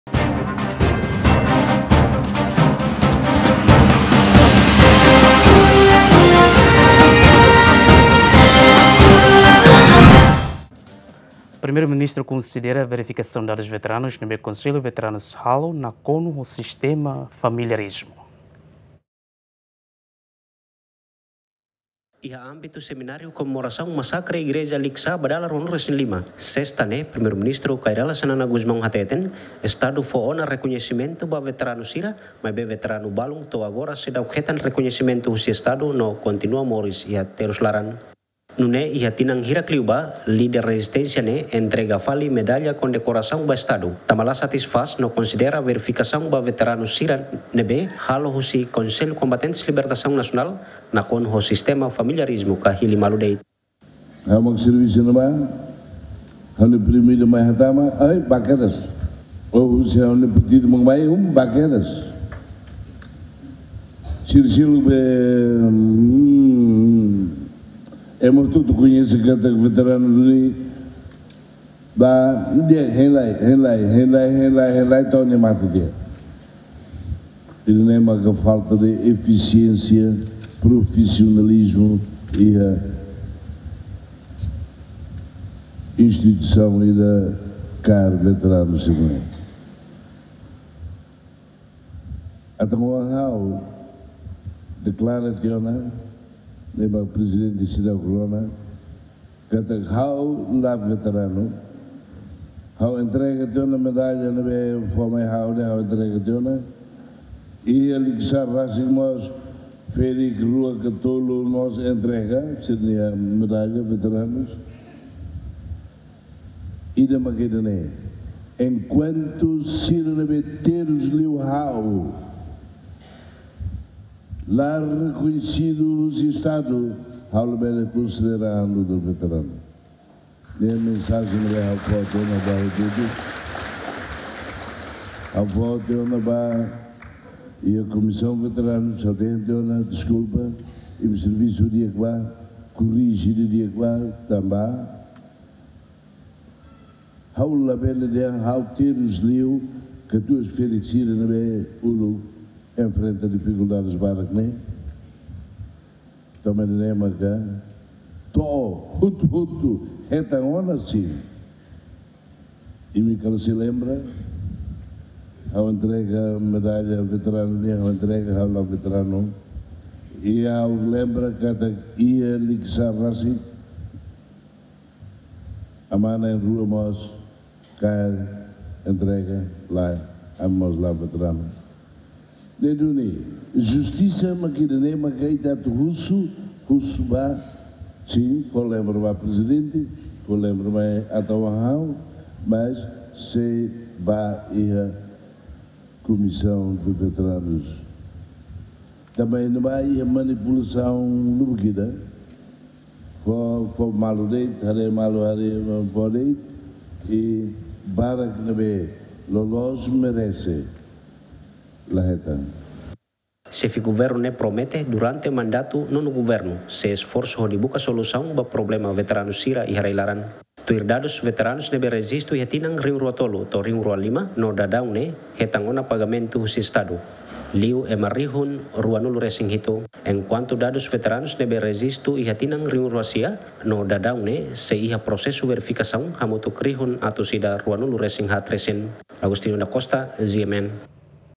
PM konsidera verifikasaun dadus veteranus nakonu ho sistema familiarizmu
Primeiru Ministru konsidera verifikasaun dadus veteranus ne'ebé Konsellu Veteranus halo nakonu ho sistema familiarizmu